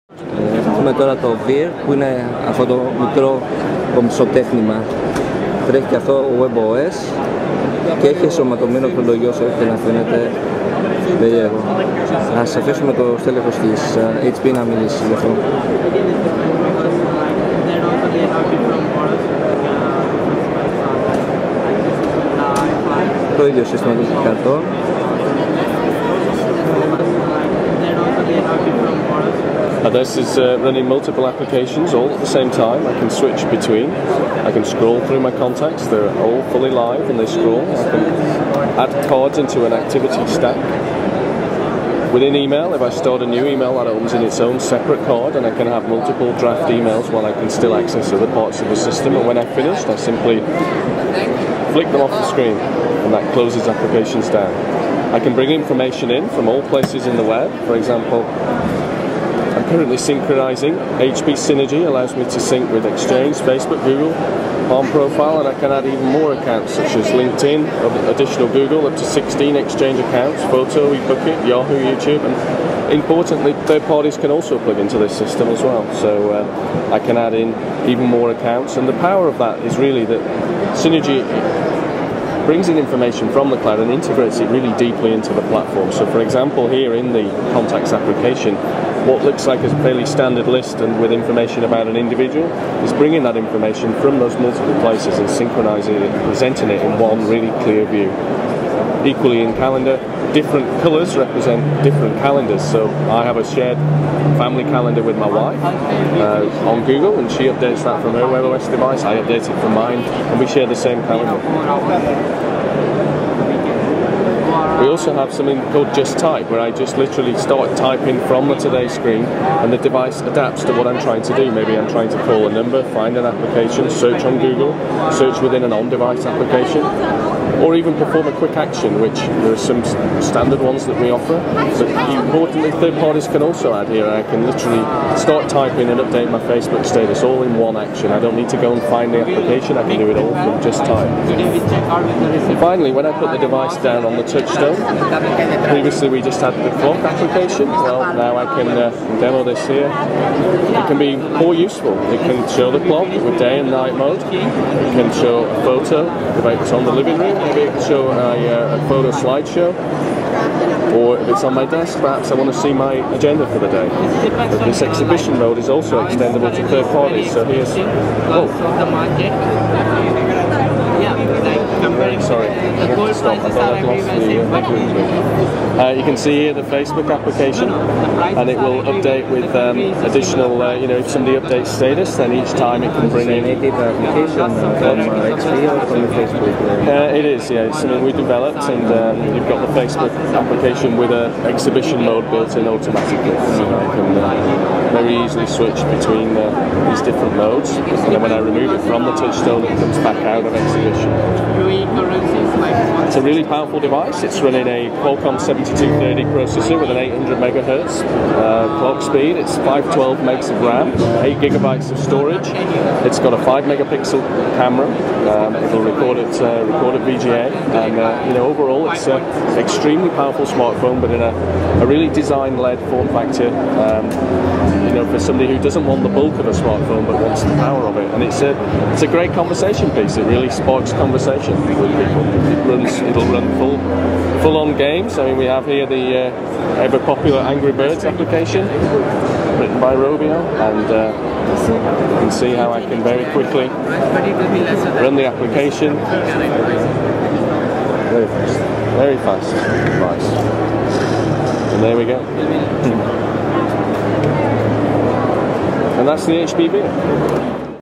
HP Veer - Το μικρό web OS smartphone
Εντυπωσιακό το μικρό smartphone της Hewlett Packard το οποίο τρέχει το web OS με όλα τα καλούδια που προσφέρει